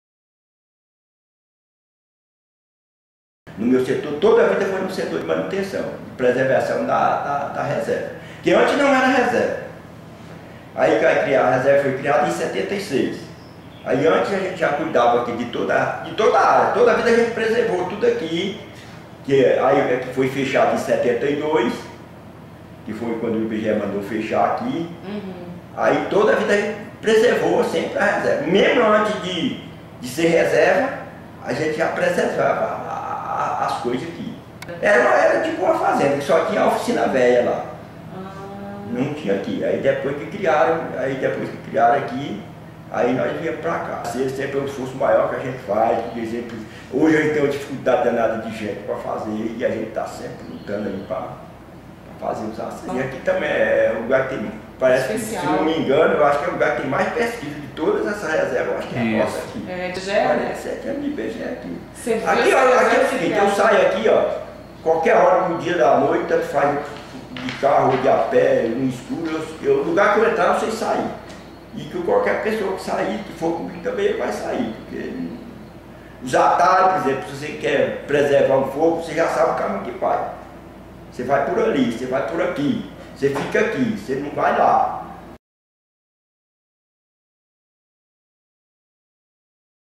Clipe da entrevista com Teodoro Pereira dos Santos - UE/DF [2015] • História Oral IBGE
Resumo da entrevista com Teodoro Pereira dos Santos, realizada como parte do projeto História Oral do IBGE em 19/08/2015 .

Saiba mais sobre a história do IBGE no site da Memória Institucional